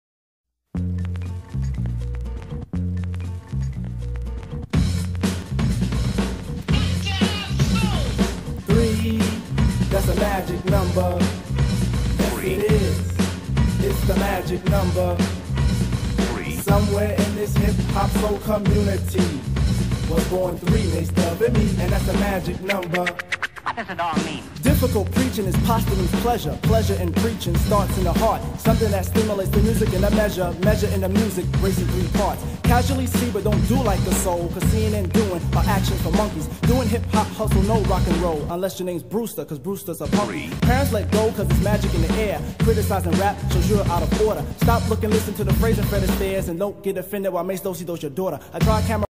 QandA annoucement vid
Hello guys! It's time for a question and answer video! Rules are in the vids Here are the links for the sites song by De La Soul